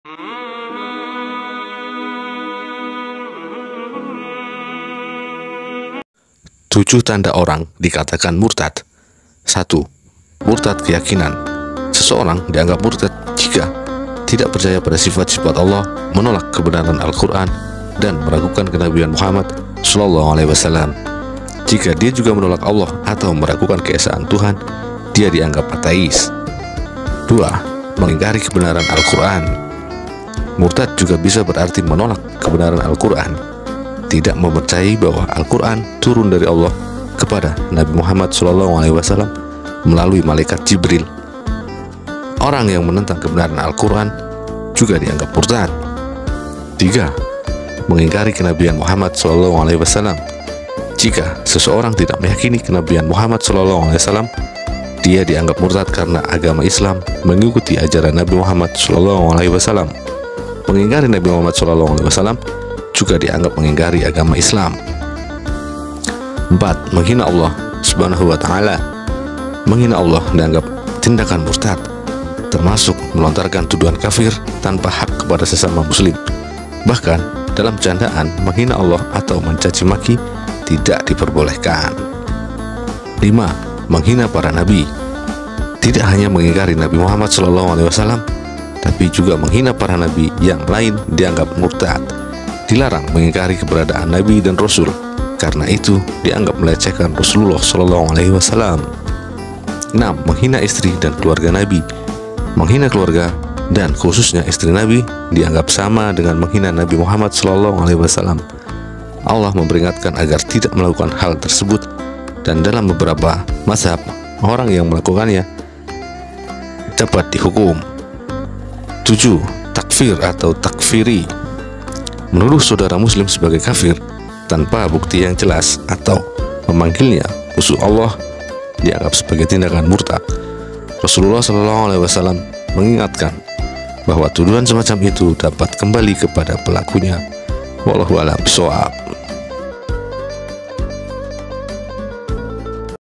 7 CIRI CIRI ORANG MURTAD DARI ISLAM || Cek Semoga Anda Terhindar
7 Ciri ciri orang murtad dari islam || Cek Semoga Anda Terhindar @Wafidot123

Murtad Keyakinan
Dalam keyakinan, seseorang dikatakan murtad ketika mengingkari sifat-sifat Allah. Dia juga menolak kebenaran Al-Quran, serta mengingkari kenabian Muhammad SAW. Ketika mengingkari Allah SWT, berarti dia seorang Atheis, tidak meyakini adanya sang pencipta.
Demikian juga dengan yang mengingkari sifat-sifat Allah. Dengan jelas dia memperlihatkan dia telah meragukan ke-Esaan Tuhan.
Hal ini bisa dilihat dari perbuatannya, bila mana dia mulai menanyakan tentang kebaradaan Allah, apakah DIA beristri, beranak, dan lain sebagainya. Termasuk juga ketika dia menolak Allah itu abadi.